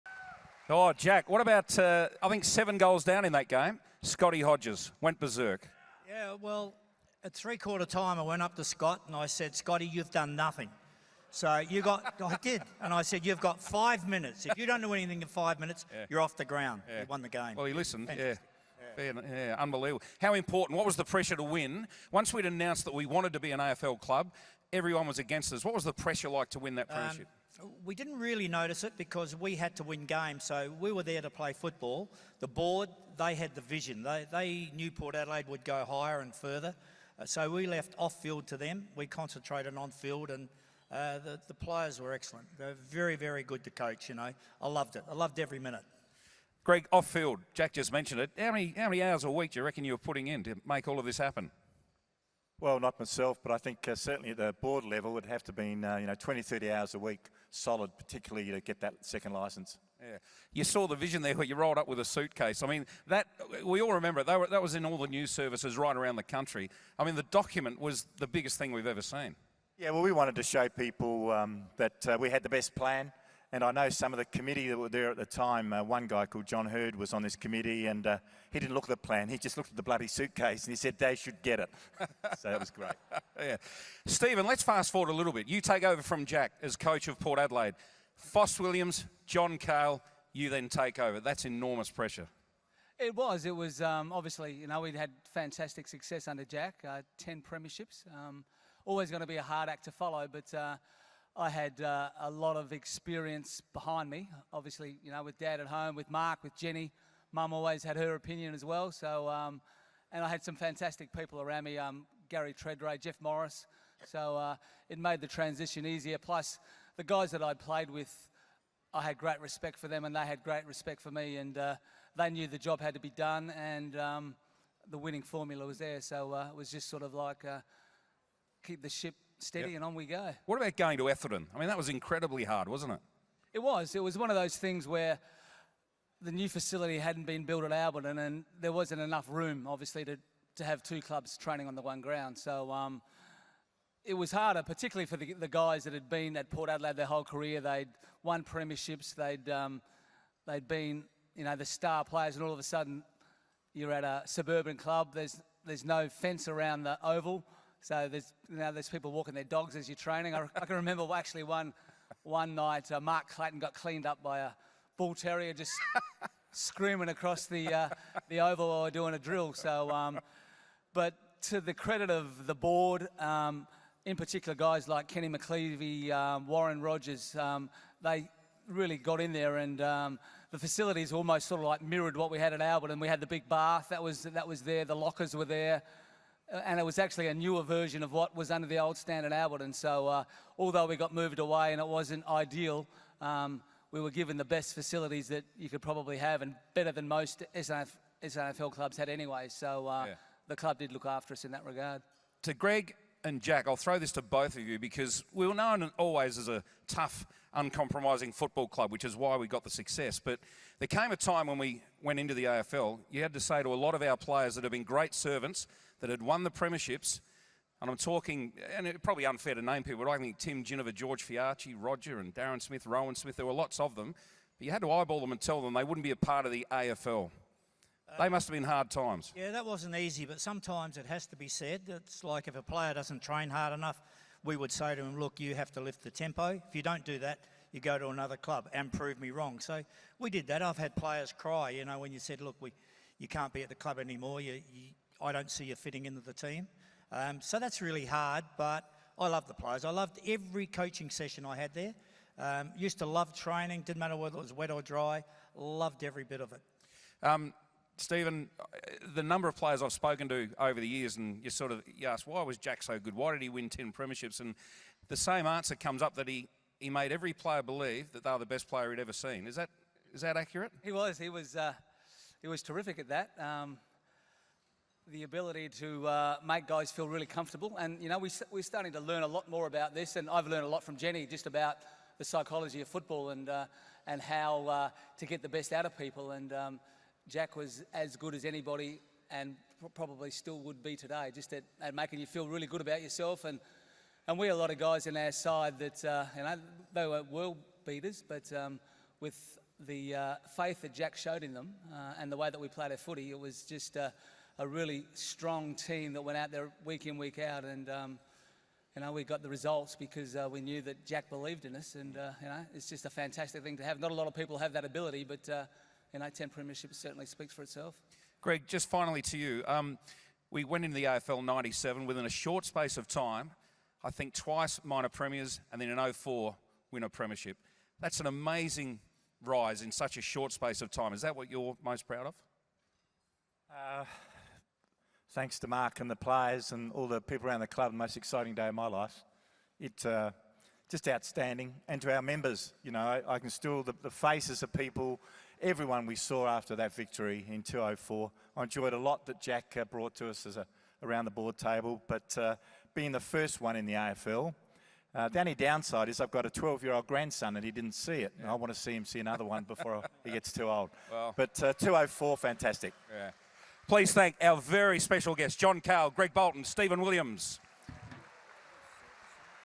150th Anniversary Gala: Journey to the AFL (Part 2)
Port Adelaide legends John Cahill, Stephen Williams and Greg Boulton discuss the club's entry to the AFL.